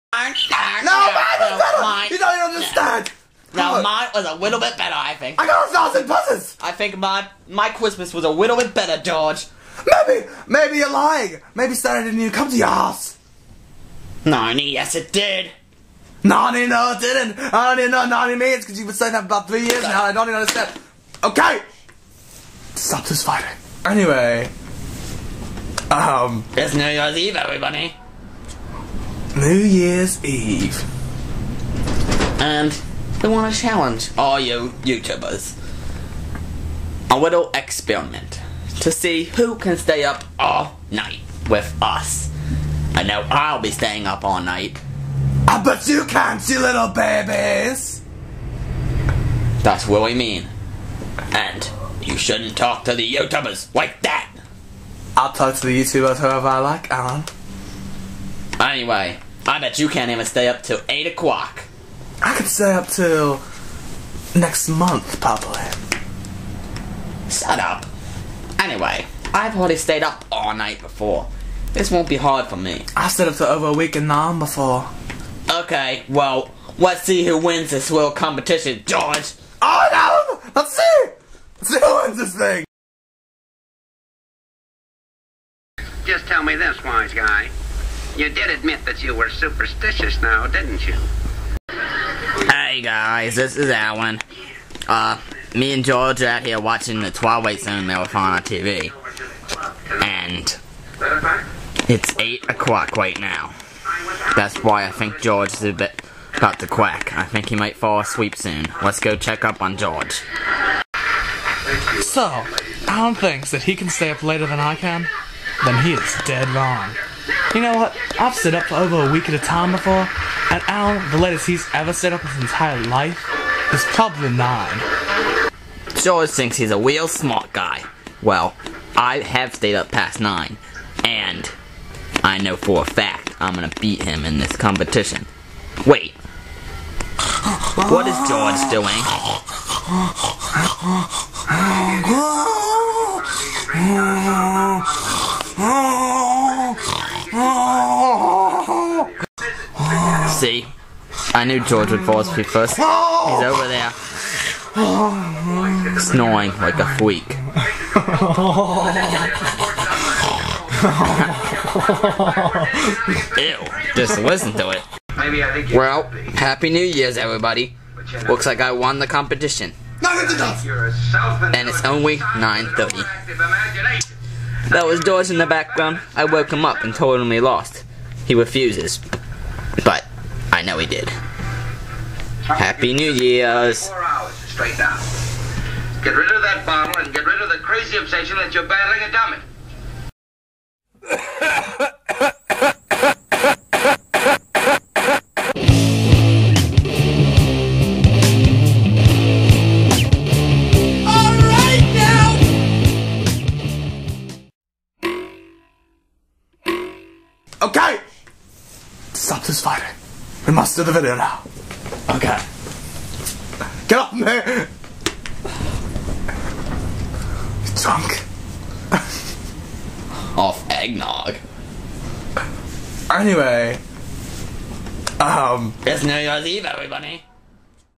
Allen and George: Staying Up All Night On New Years Eve
Allen and George have a competition to see who can stay up the latest on New Years Eve.